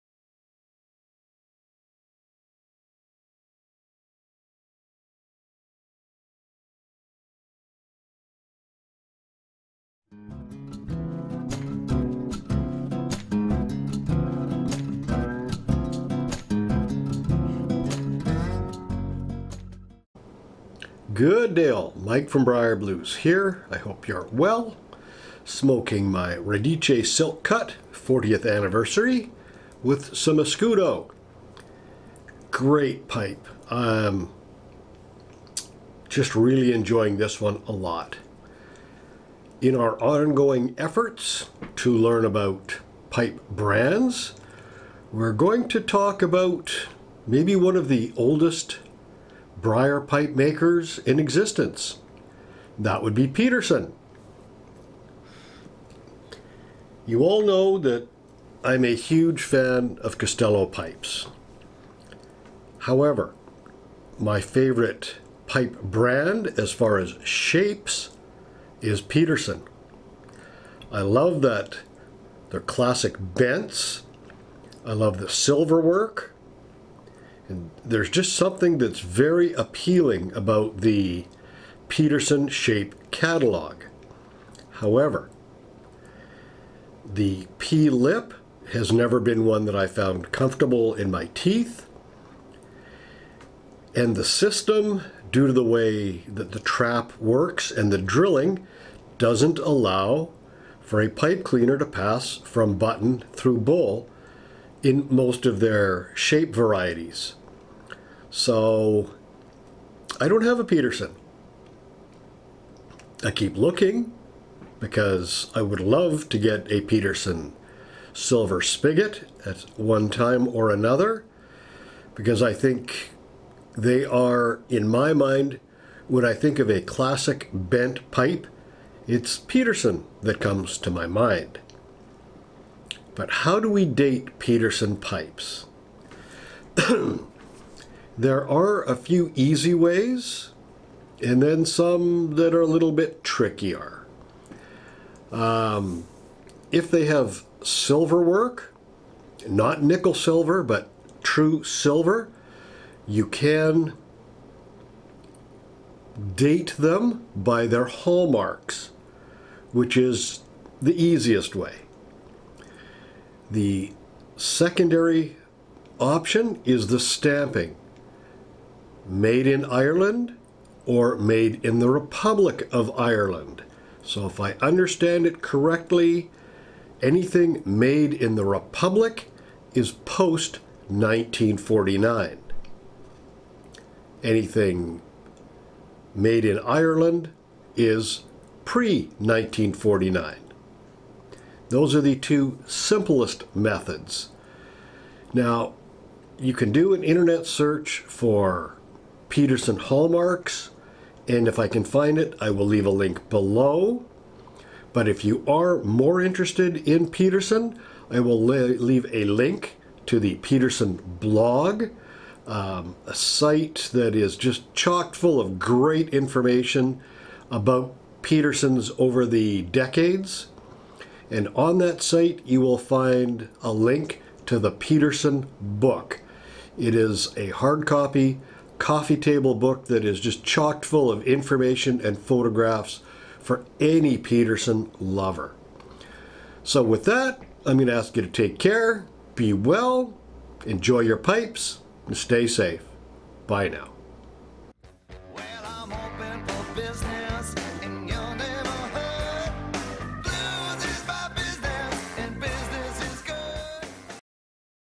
The Thinking Man's Pipe - Peterson
Today let’s look at one of oldest briar pipe brands, that being Peterson.  This is a brand that has been very divisive over the years.   It’s almost either love or hate, as far as collectors.  Personally I love the shapes, but the P Lip, system trap with high draft hole drilling is a deal breaker.  The only two shapes that appear to allow a pipe cleaner to pass from button to bowl, on a regular basis are …. The 999 bent Rhodesian, and the 106 or is it 109 … straight billiard.

 If Peterson is a brand you have interest in, here are two url’s that you might find of interest.